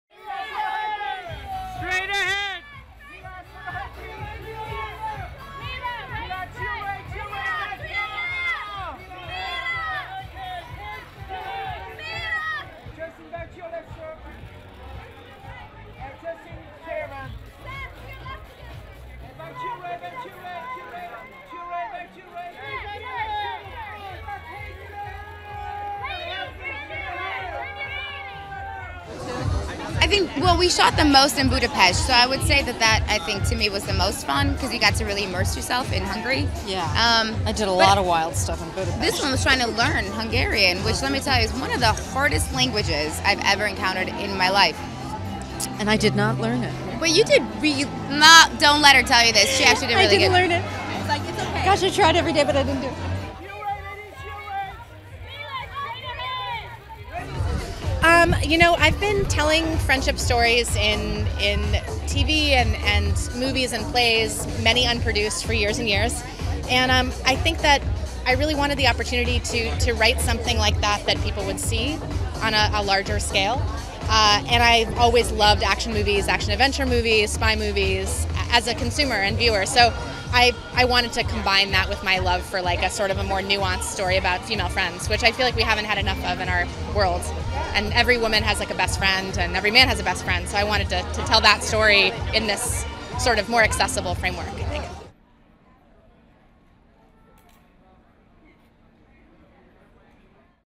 EVENT CAPSULE CLEAN - The Spy Who Dumped Me World Premiere
EVENT CAPSULE CLEAN - The Spy Who Dumped Me World Premiere in Los Angeles, CA 7/25/18

Featuring:
    Mila Kunis
    Kate McKinnon
    Justin Theroux
    Sam Heughan